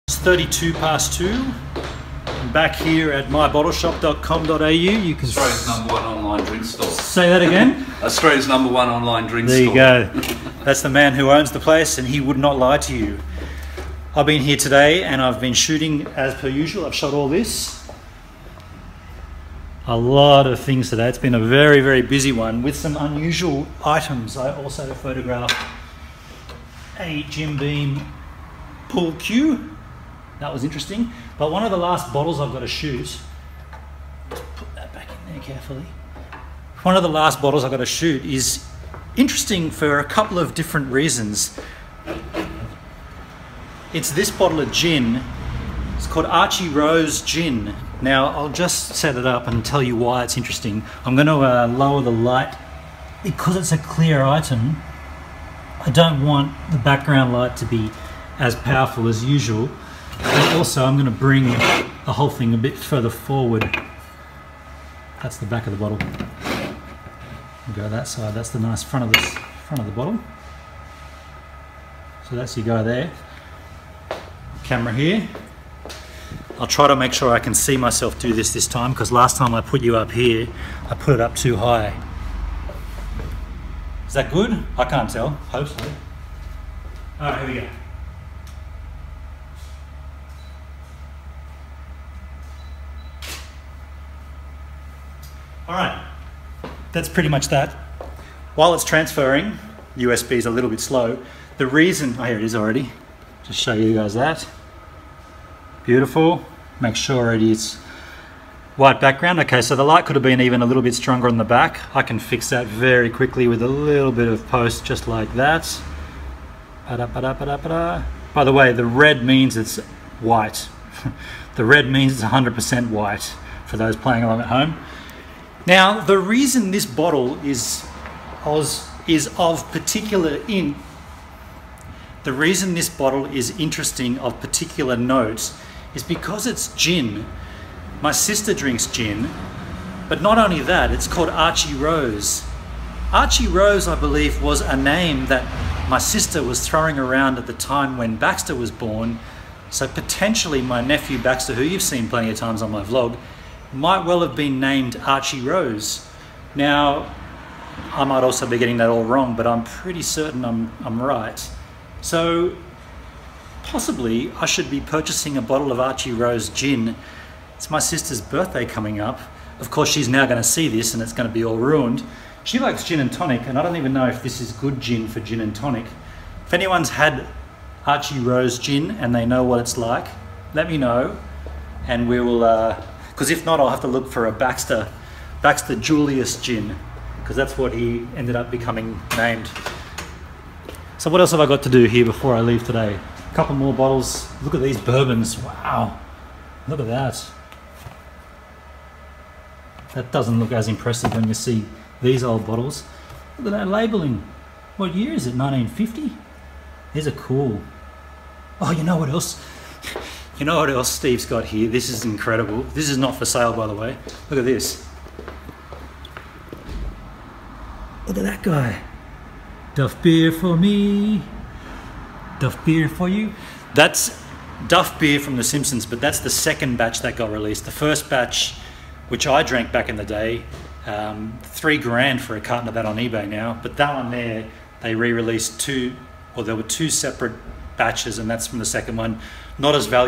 Archie Rose Gin Is Such A Good Name
At one point my sister was thinking of naming her son Archie Rose, I think it is such a good name even for a Gin! If you're a photographer you may find my Lightroom Presets handy

This is daily vlog 219 since I began this channel in August 2015 and today between 14:32 and 14:36 I was at mybottleshop.com.au shooting a bottle of Archie Rose Gin and wondering if this is where my sister got the name from?